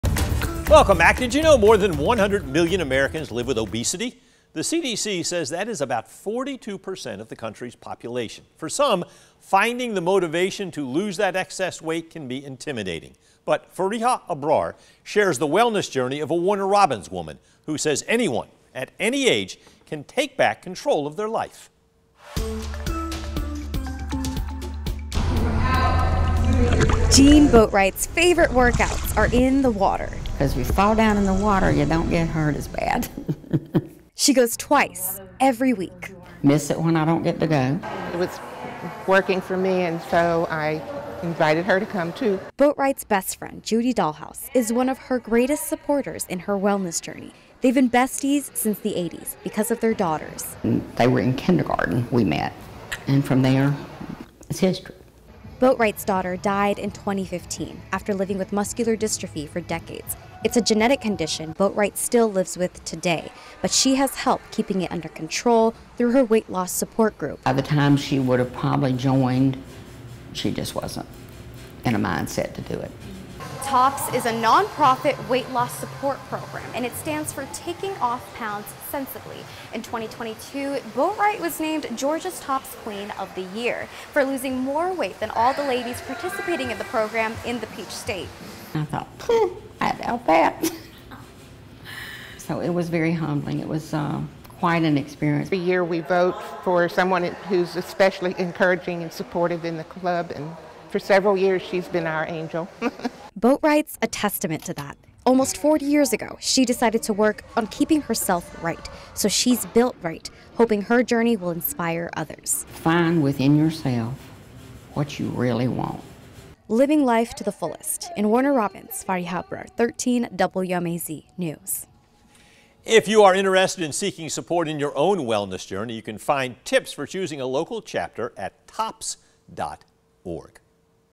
Warner Robins woman shares her 40-year wellness journey after honored with weight loss award
Jean Boatright was named Georgia's 2022 TOPS Queen through her weight-loss support group, "Taking Off Pounds Sensibly."